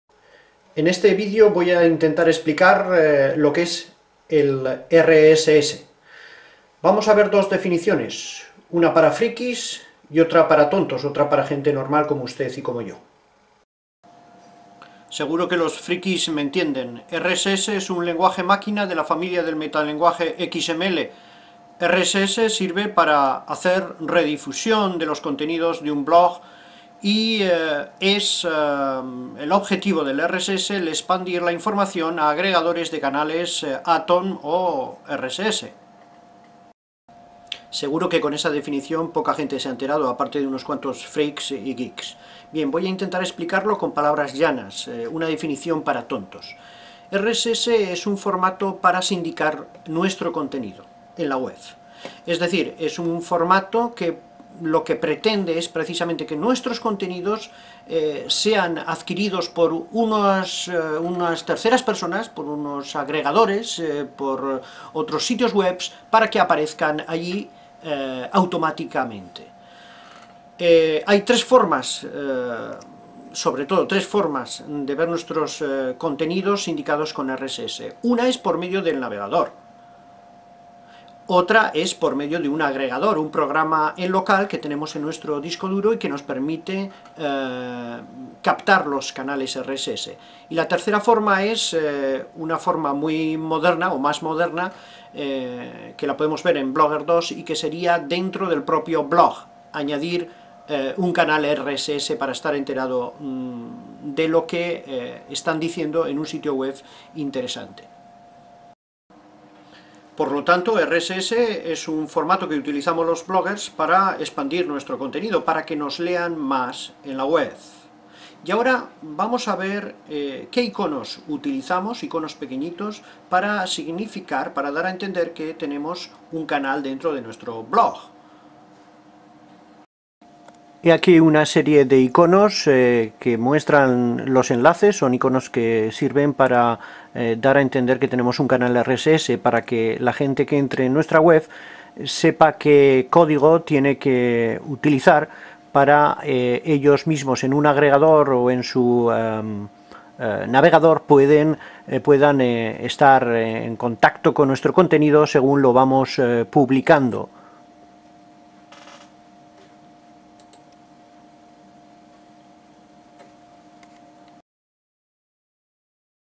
Definición de RSS
El profesor de la Universidad del País Vasco Gorka Jakobe Palazio explica qué es el RSS. Nos da dos definiciones: una para geeks y otra para tontos. El vídeo es parte de la factoría de contenidos audiovisuales Eduvlogs en la URL